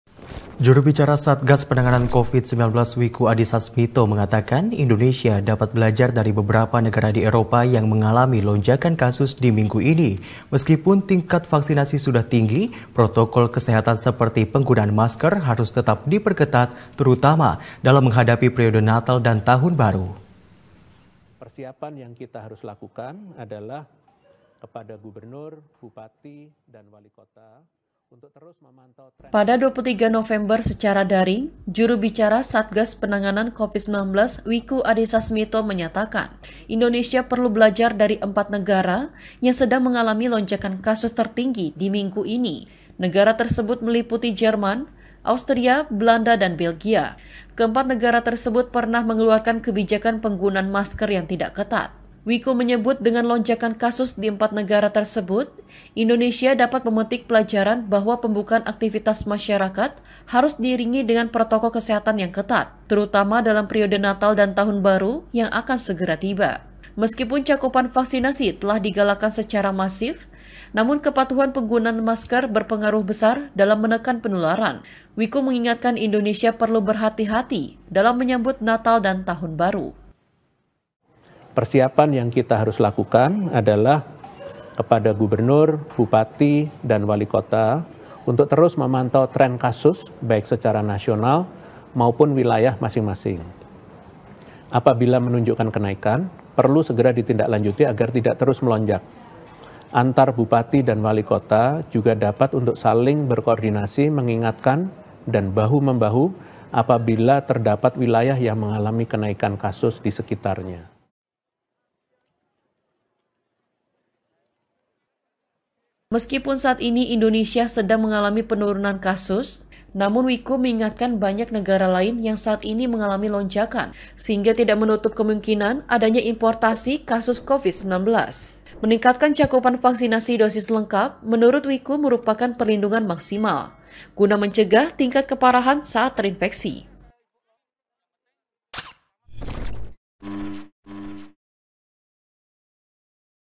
Tingkat Vaksinasi Sudah Tinggi, Prokes Harus Diperketat. Wiku: Indonesia Dapat Belajar Dari Eropa
- Pada 23 November secara daring, Juru Bicara Satgas Penanganan Covid -19 Wiku Adisasmito menyatakan, Indonesia perlu belajar dari empat negara yang sedang mengalami lonjakan kasus tertinggi di minggu ini. Negara tersebut meliputi Jerman, Austria, Belanda dan Belgia.